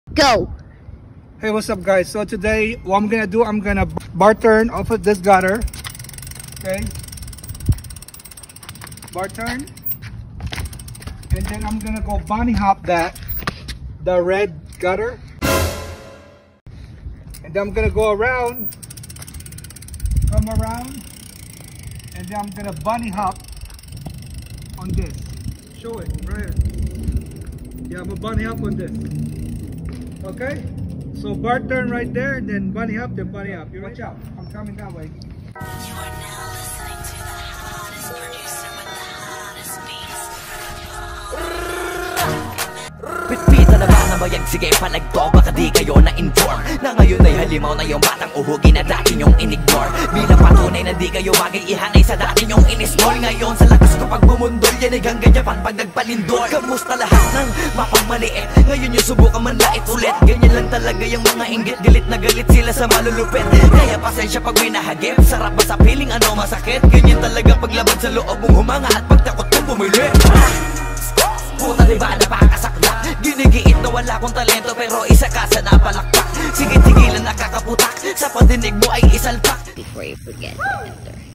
Bunny Hop mtb over 40 (Watch My Old Ass)..
I just learned how to Bunny Hop at the age of 43.  Watch me Bunny Hop Off the Gutter and Do some Bar Turn..